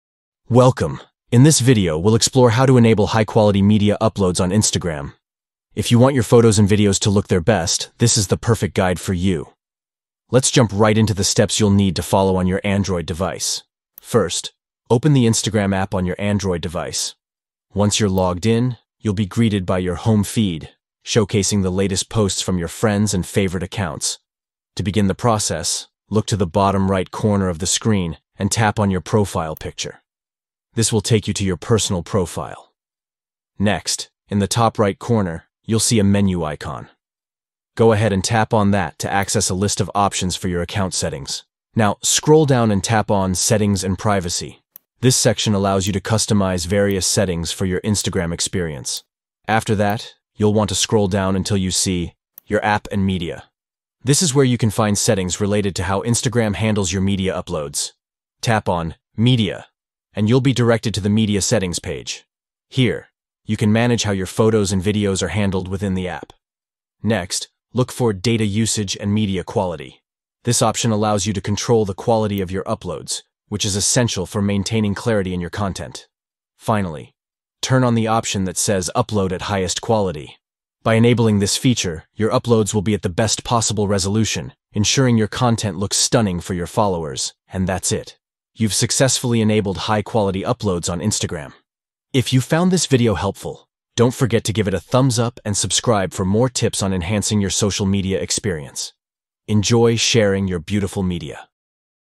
How to Enable Upload High Quality Media On Instagram (Updated)
Learn how to enable upload high-quality media on Instagram with this updated guide! Follow these simple steps to enhance your media quality on your Android device:

1. Open Instagram app on your Android device.
2. Tap your profile picture at bottom right.
3. Tap the menu icon at top right.
4. Tap "Settings & Privacy".
5. Scroll down then under "Your app and media".
6. Tap on "Media".
7. Tap "Data usage and media quality".
8. Turn on "Upload at highest quality".

Now your photos and videos will look their best.
Chapters:
00:00 Intro
00:12 Soultion
01:05 Outro